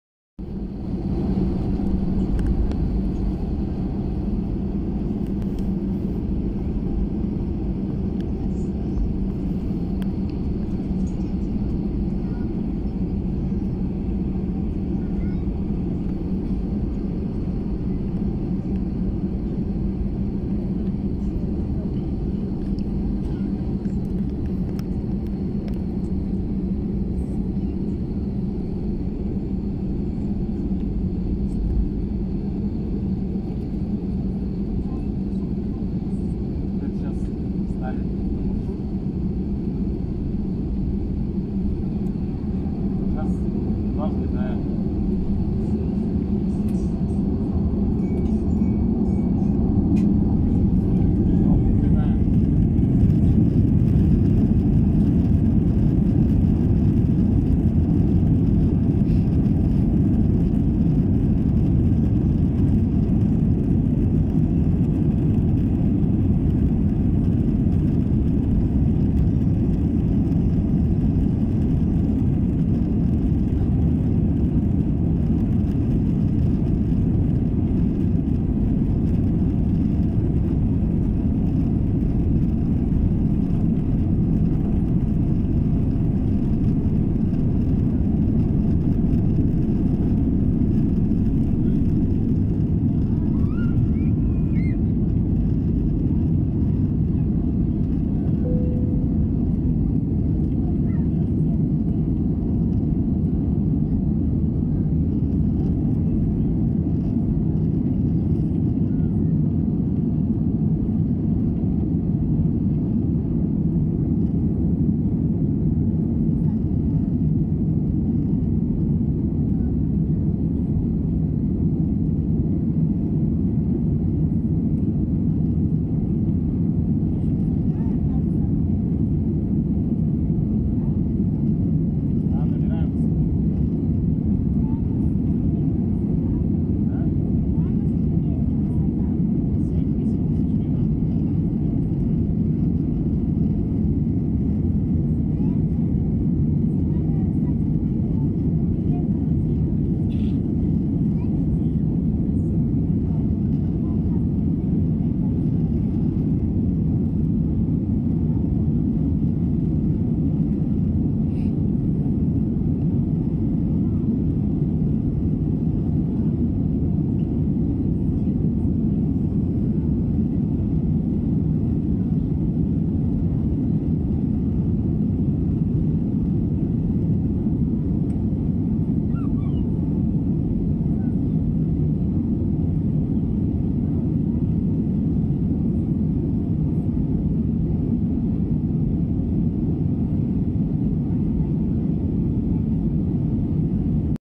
Взлёт в Екатеринбурге (Кольцово) / Taking off in Yekaterinburg (Koltsovo International Airport)
Рейс Екатеринбург-Москва, а/к S7, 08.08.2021, Airbus A320
Flight Yekaterinburg-Moscow, a/c S7